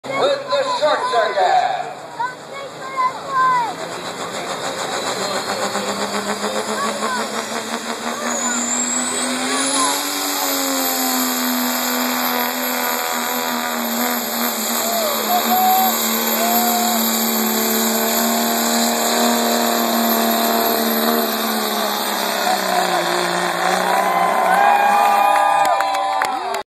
Short Circuit
Darin Morris with Short Circuit
WNY Pro Farm Pullers
Langford, NY